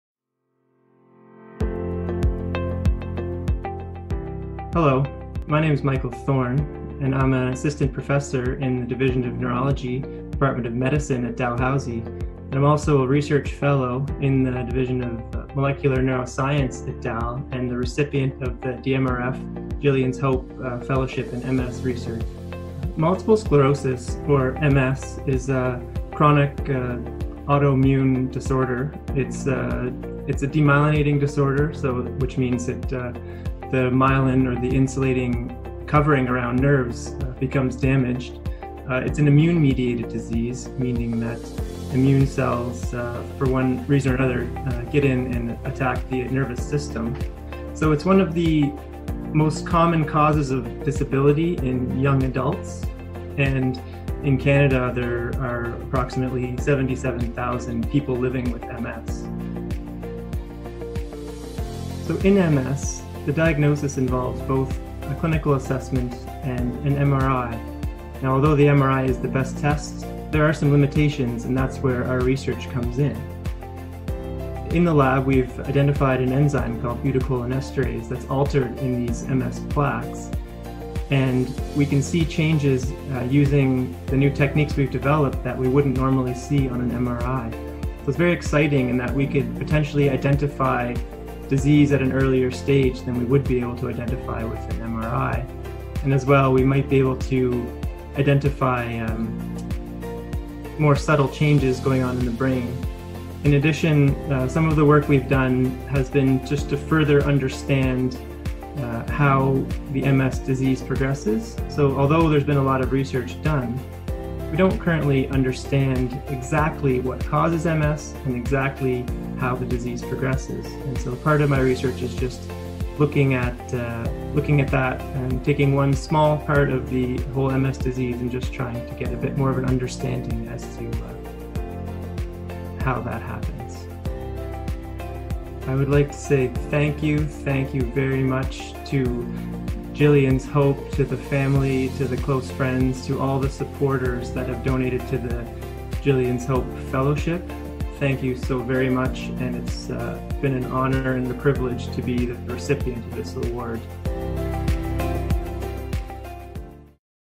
Dr. Michael Thorne
“I’m honoured to be the first recipient of the DMRF Gillian’s Hope Clinical Fellowship in MS. I plan to continue MS research until we fully understand what causes this disease, and hopefully one day find a cure. This Fellowship will support a long line of people like myself, working toward the same goal.”  - Dr. Michael Thorne